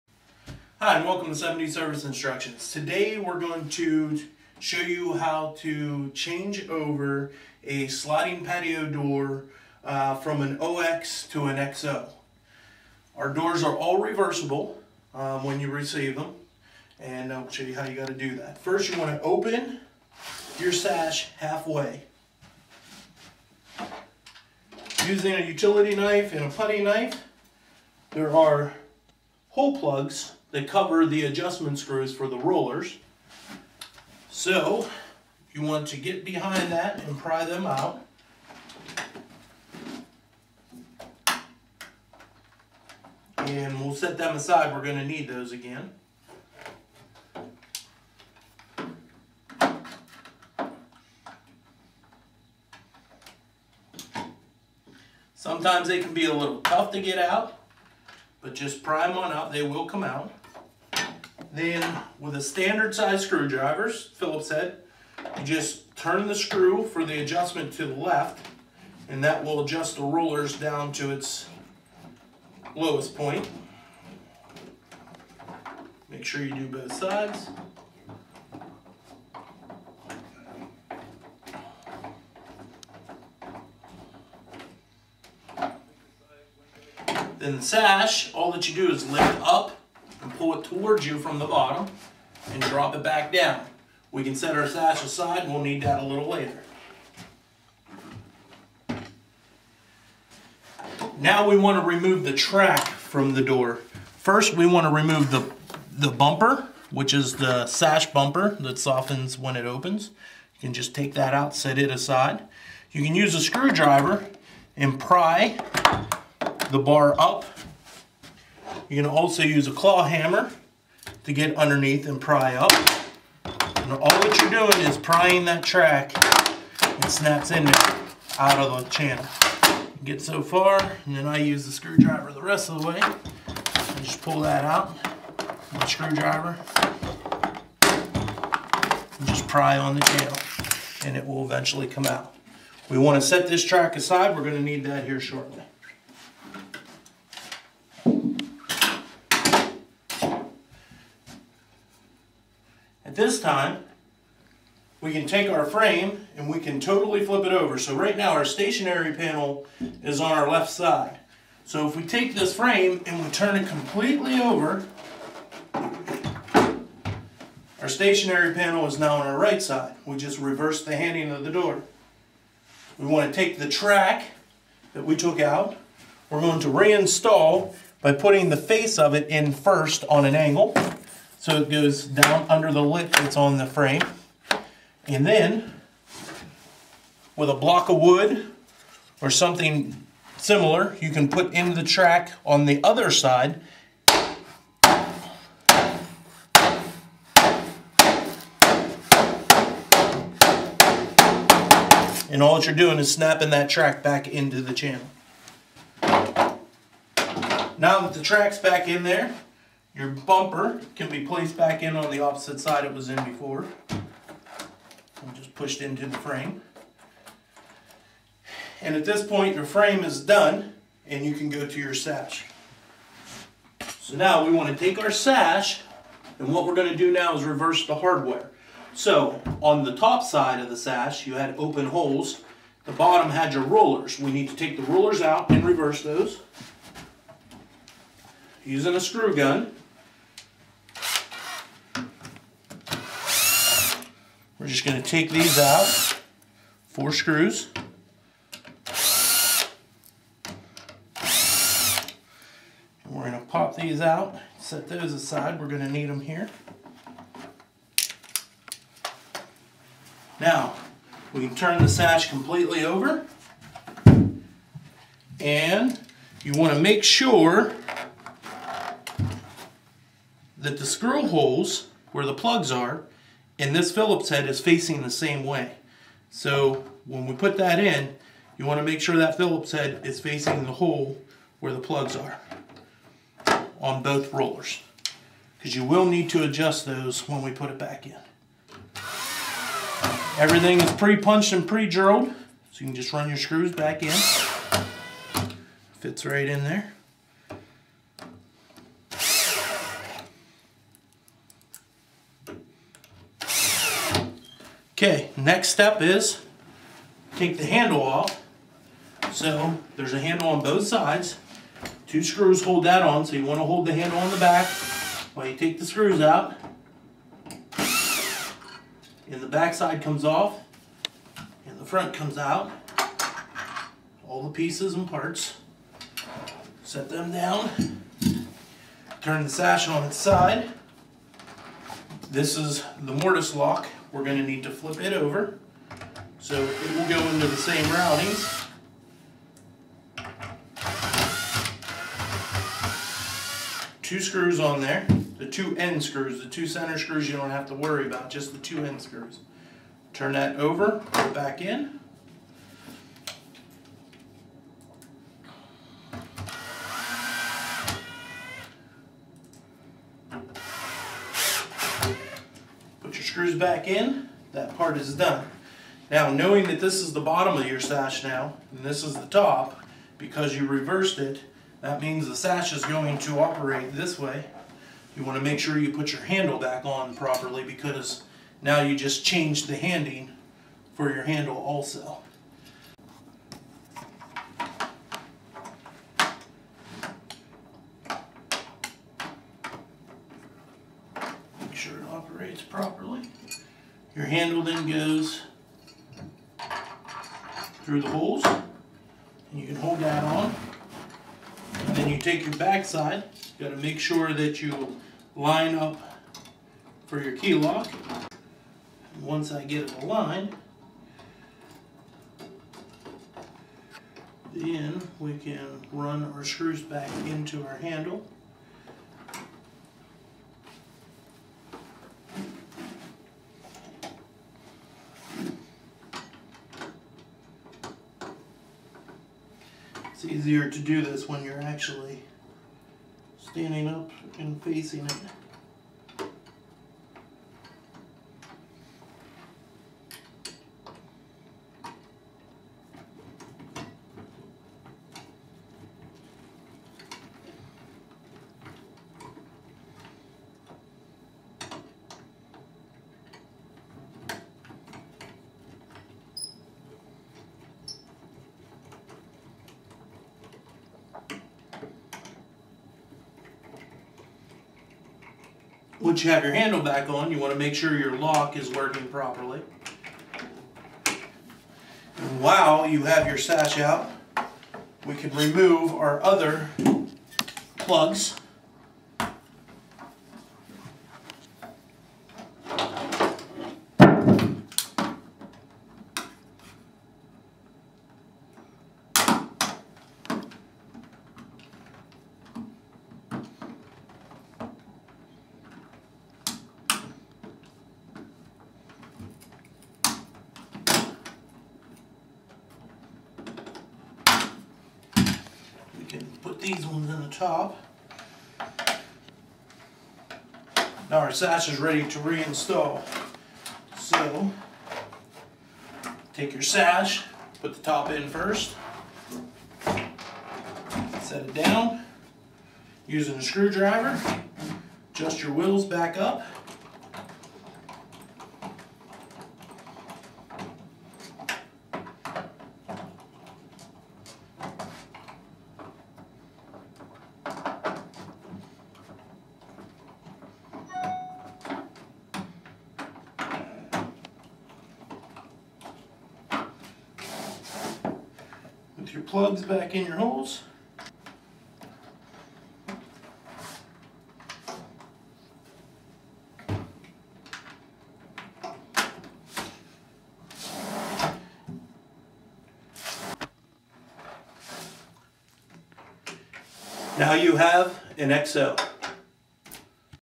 How to reverse the handing of a patio door
Seven D Industries General Manager, Mike Pettenati, demonstrates how to reverse the handing of a 7D Industries 7evolution patio door.